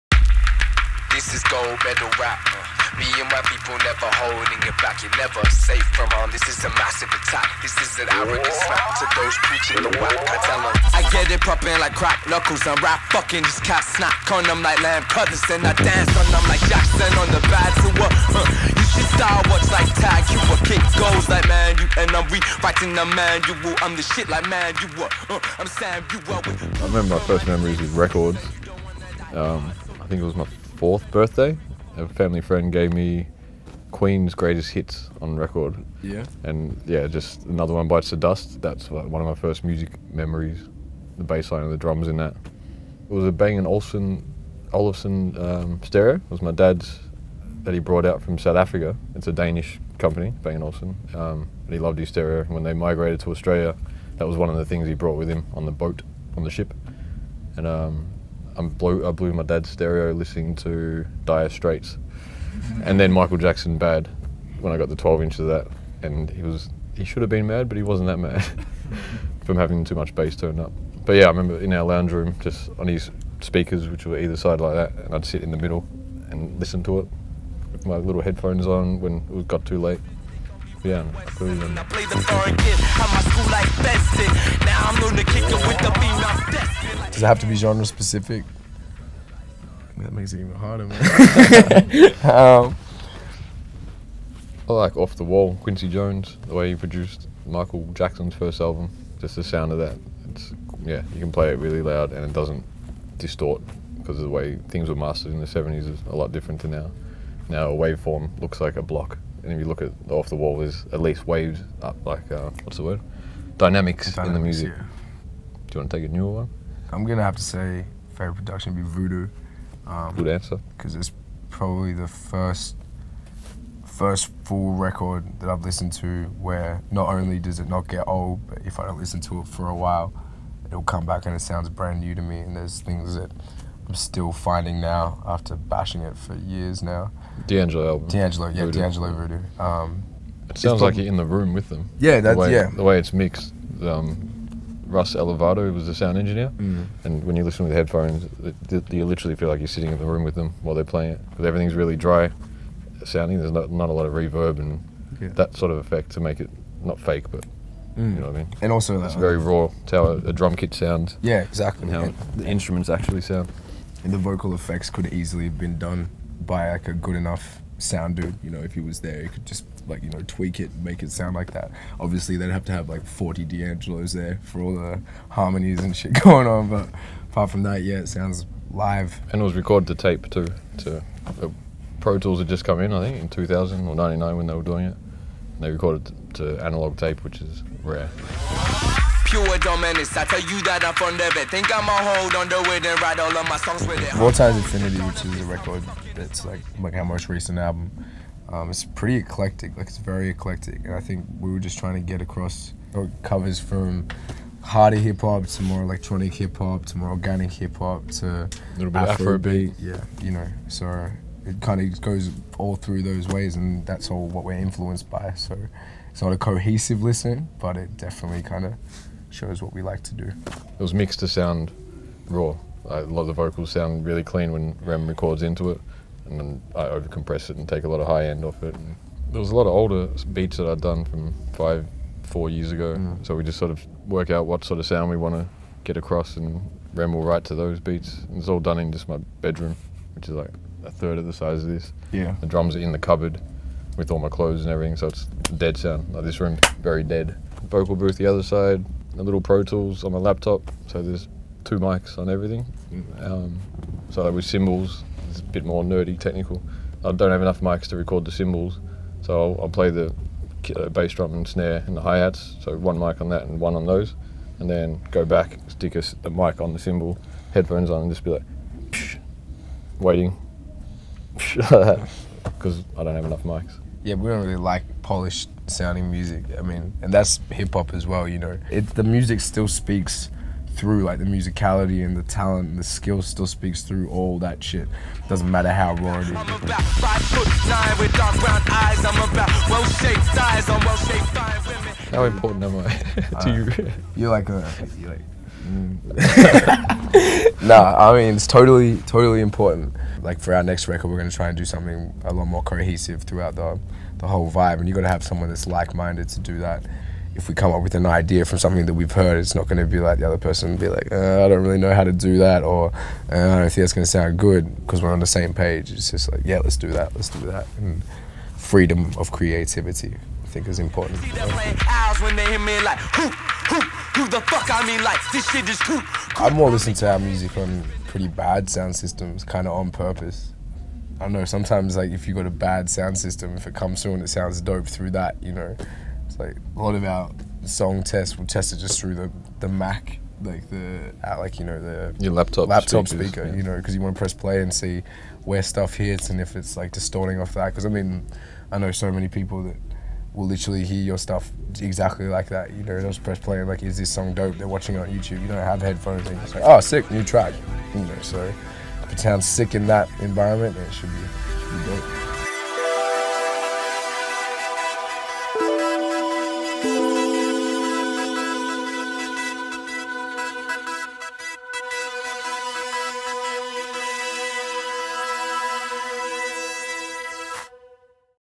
Hi-Fi Scenen Talks - REMI
At the WOO HAH! Festival in Tilburg we sat down with REMI to talk about sound. They told us their story on producing and sound engineering, about their favorite items in the studio, about their way of listening to music and much more. Enjoy!